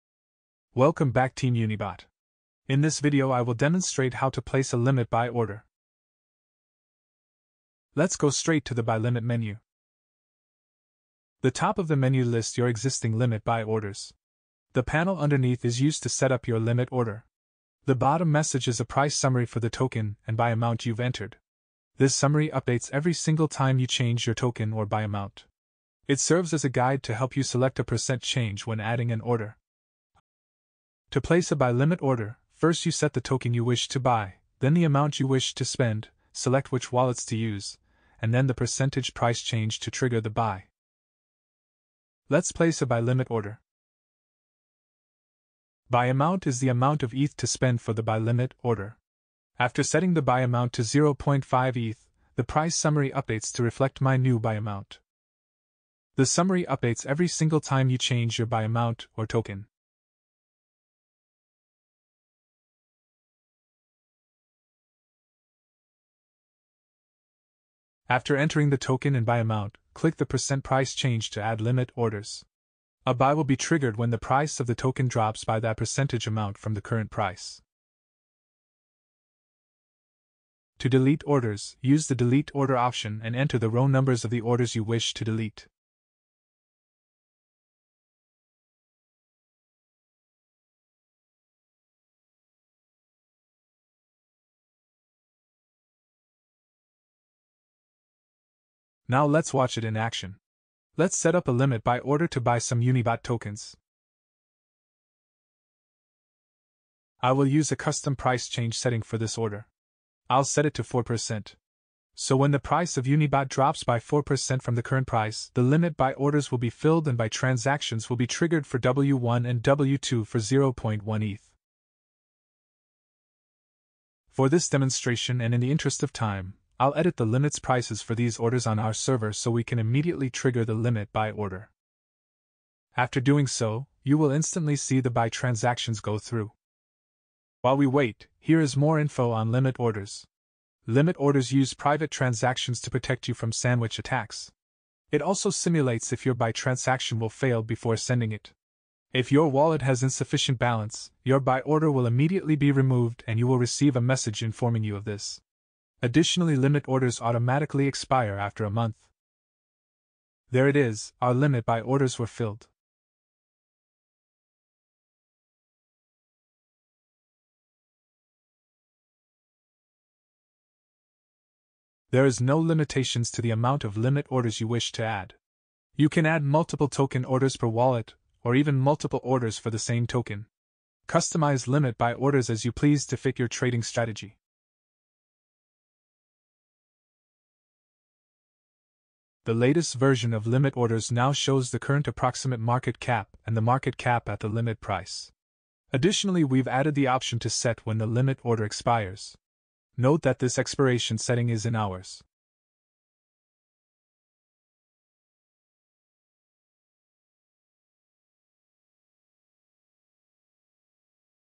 Tutorial #8: Limit Buy Orders
Place limit buy orders to catch the dips for your favorite tokens. There are no limitations to the amount of orders you place or the type of orders.

Check Unibot X - The Ultimate on-chain Trading Terminal. Start trading